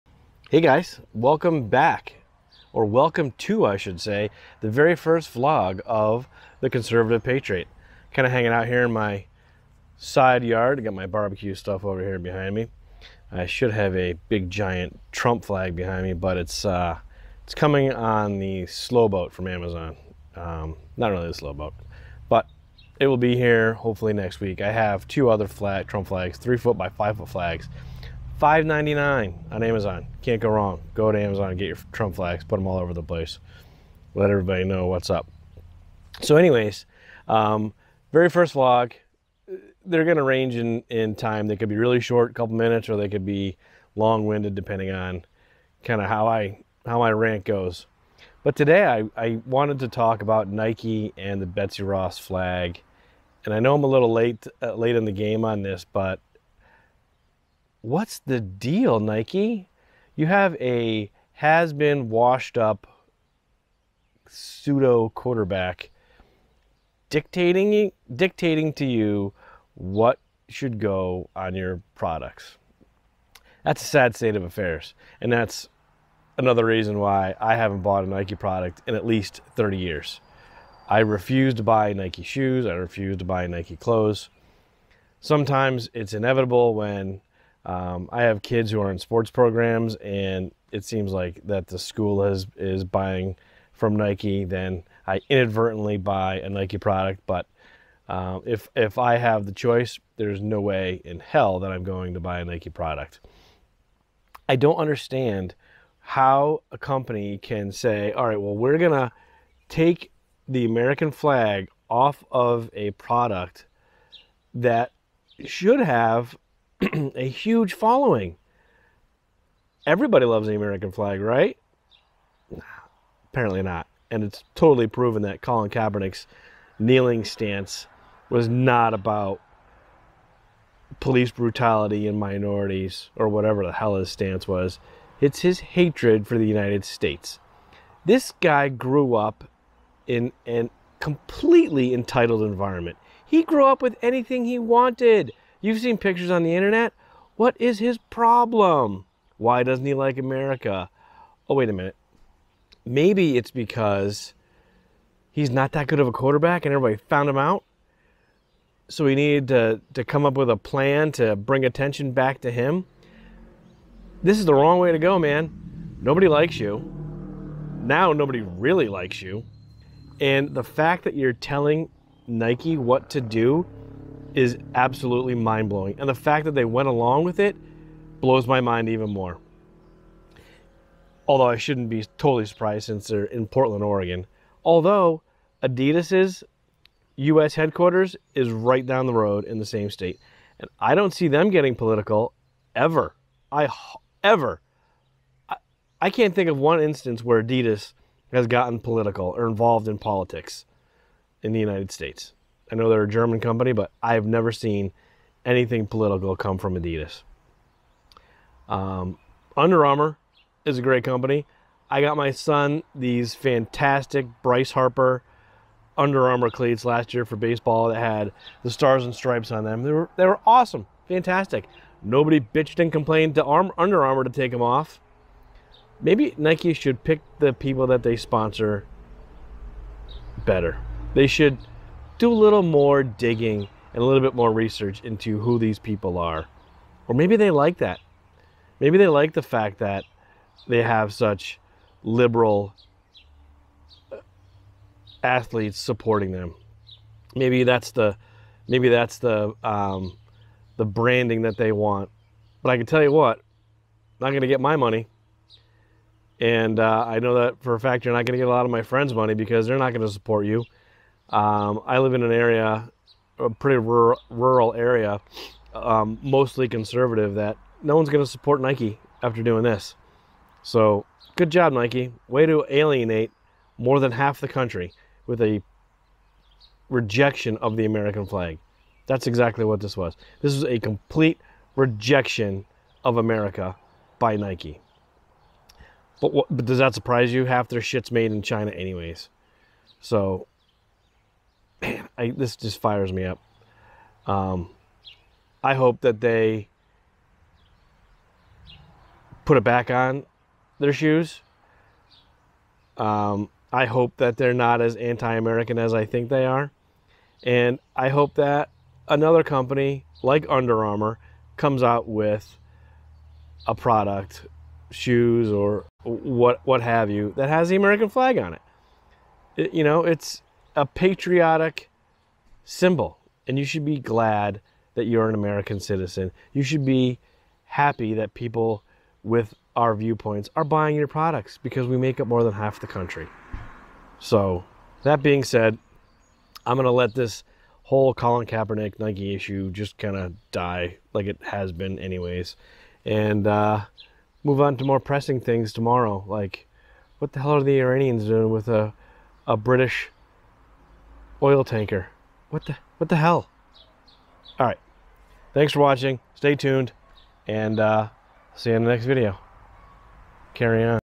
Nike, Colin Kapernick, Besty Ross Flag
my take on Nike, colin kapernick and the betsy ross flag.